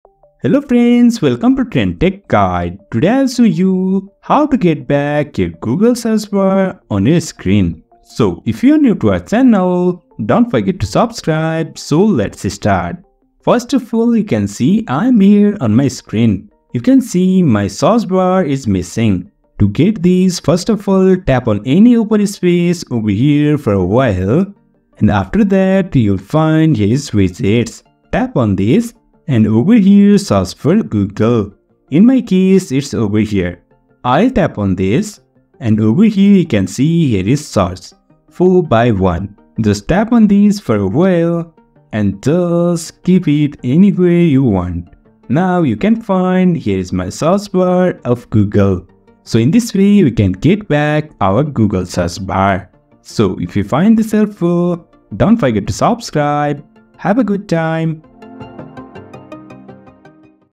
How to Get Back Your Google Search Bar | Restore Missing Google Search Bar on Android
Is your Google Search Bar missing from your Android home screen? In this video, I'll guide you through the steps to get back your Google Search Bar quickly and easily. Follow along as we restore the missing Google Search Bar so you can start searching the web effortlessly again. Whether you accidentally removed it or it's disappeared after an update, this tutorial will help you get it back.

👉 Explore more!